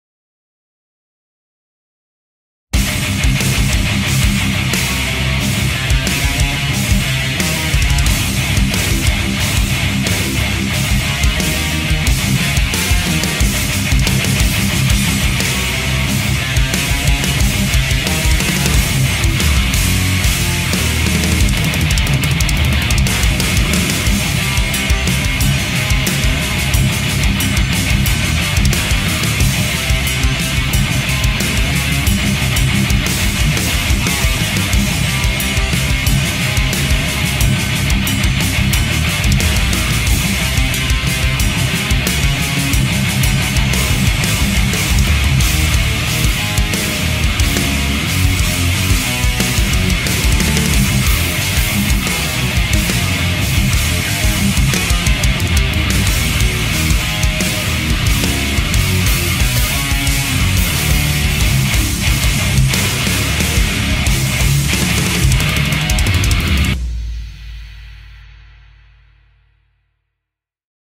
Bogren Digital July Contest
Recorded with Mesa Boogie Triple Rectifier, FORTIN 33 pedal Bogren Digital Erectifier IR and Jackson KV Pro.